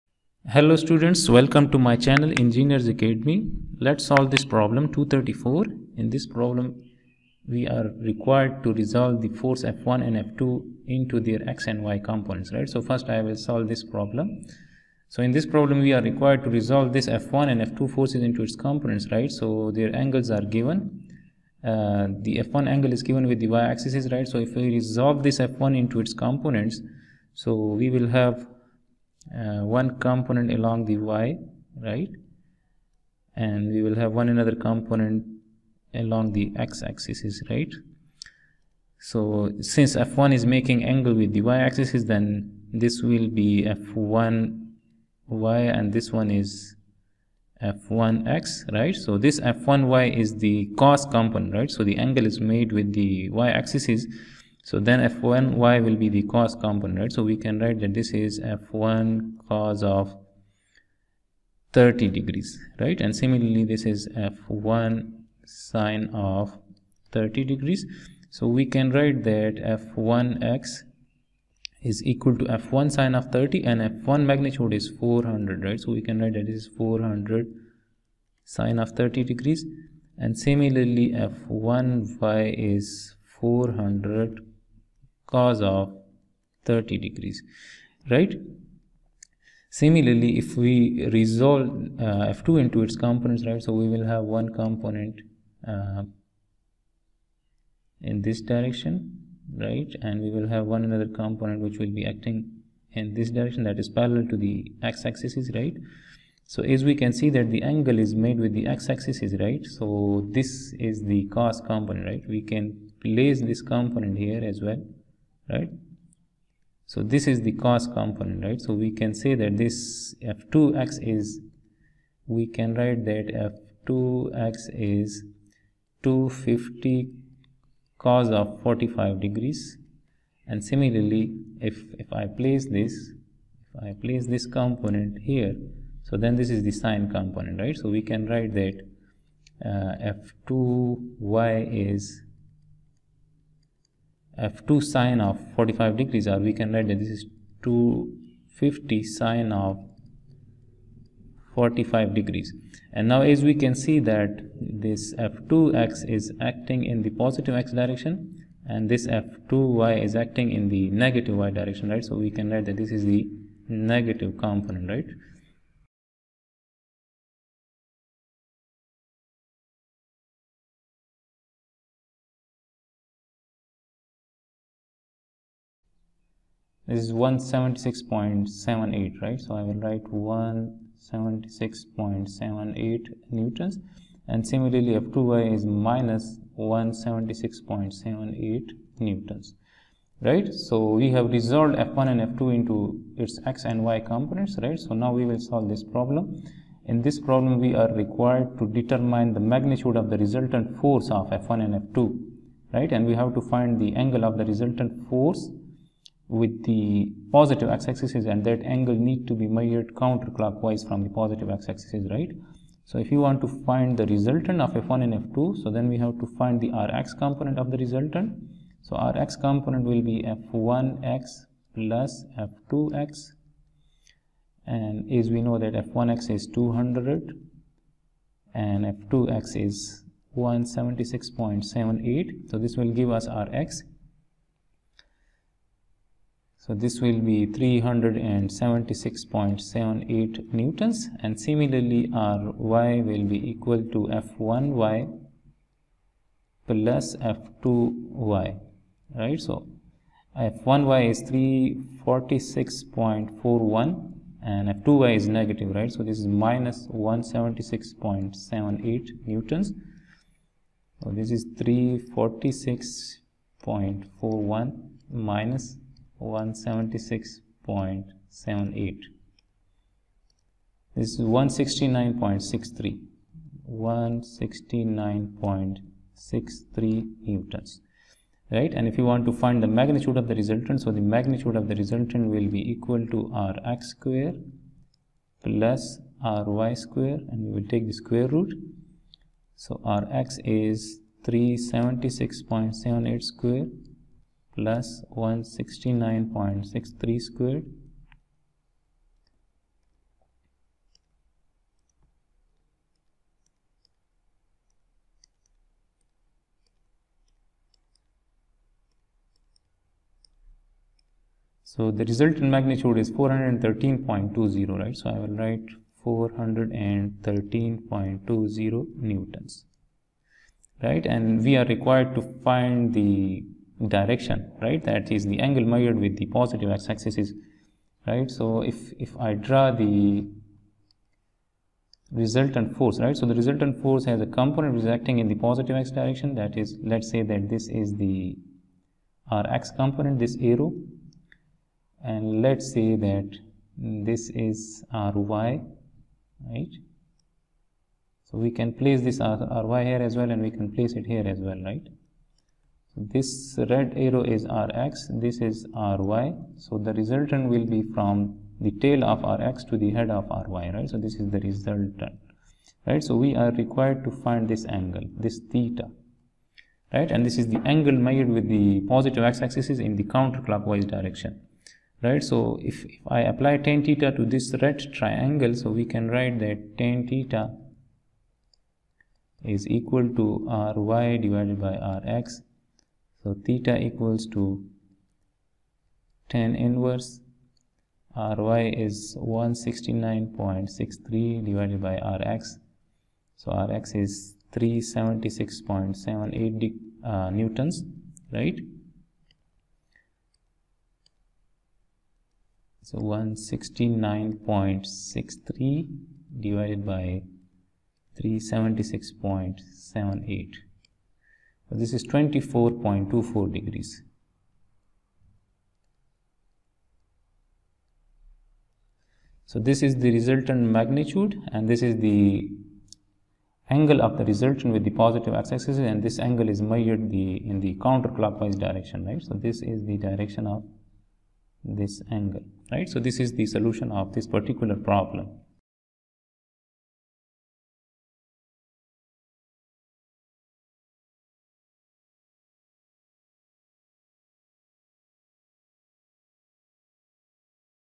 Determine the magnitude of the resultant and its direction | Hibbeler Statics | Engineers Academy
2-34,35 | Hibbeler Statics 14th Edition (Chapter 2) | Engineers Academy
Chapter 2: Force Vectors

2–34. Resolve F1 and F2 into their x and y components.
2–35. Determine the magnitude of the resultant force and its direction measured counterclockwise from the positive x axis.